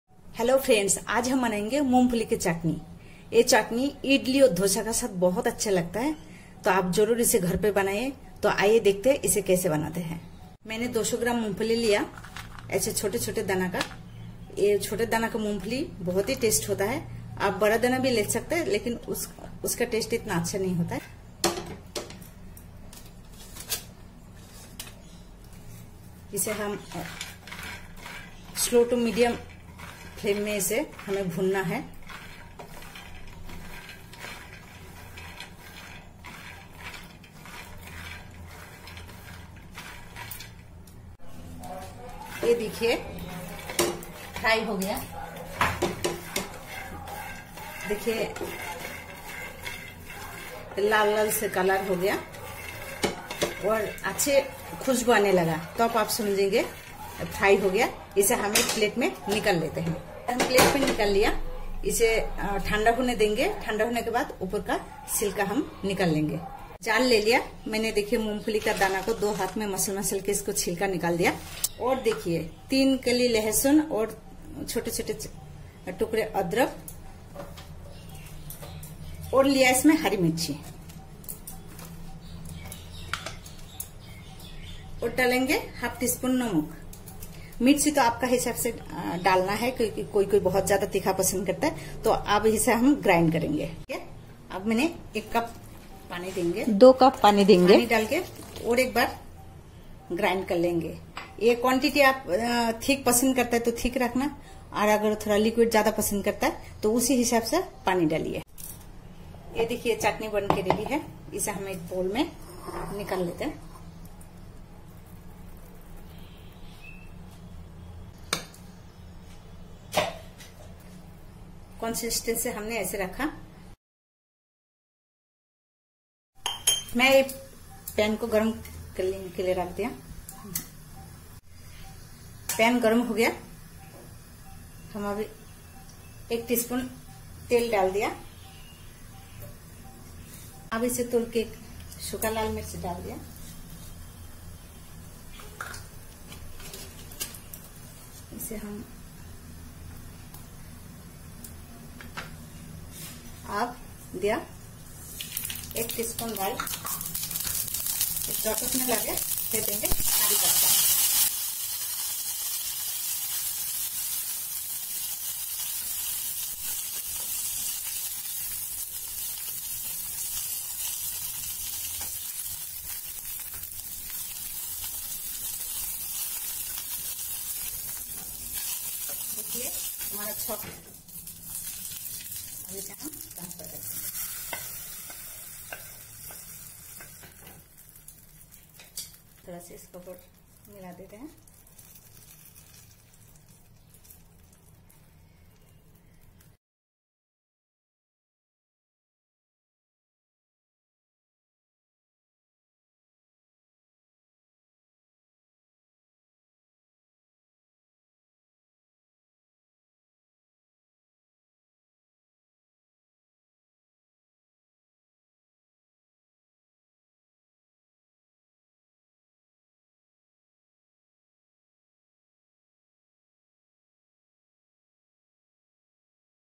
Moongfuli Chutney | Peanut Chutney Recipe in Hindi | Chutney for Idli and Dosa
Today, I will be preparing Peanut Chutney. It is served with Idli, Dosa and Uttapam. I have prepared this chutney with the ingredients that are easily available at home.